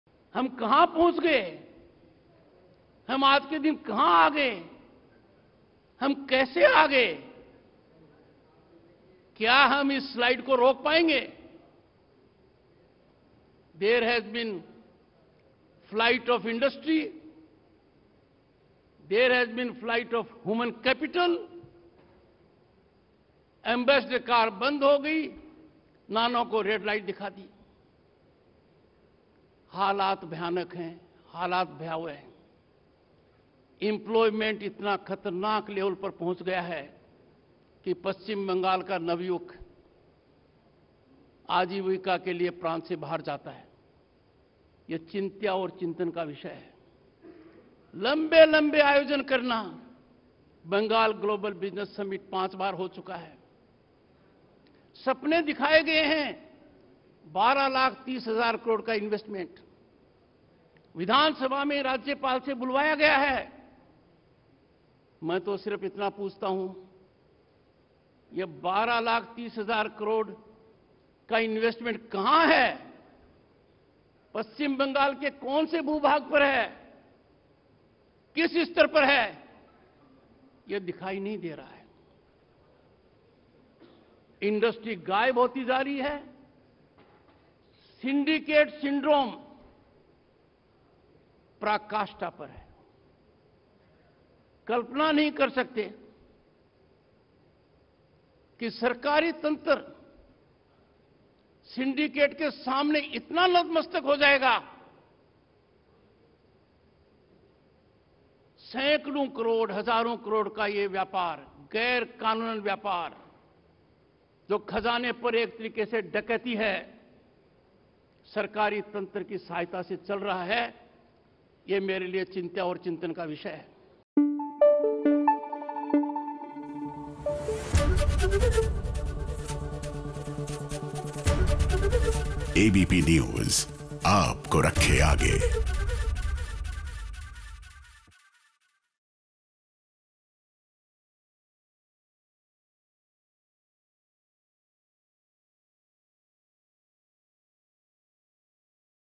West Bengal में बंद हो रहे उद्योग-धंधे, राज्य से बाहर जा रहे युवा: Jagdeep Dhankhar
ABP Shikhar Sammelan में बंगाल के राज्यपाल Jagdeep Dhankhar ने ममता सरकार पर निशाना साधते हुए कहा कि राज्य में उद्योग-धंधे बंद हो रहे हैं, प्रदेश का युवा अजीविका के लिए दूसरी जगह जा रहा है. ये चिंता का विषय़ है.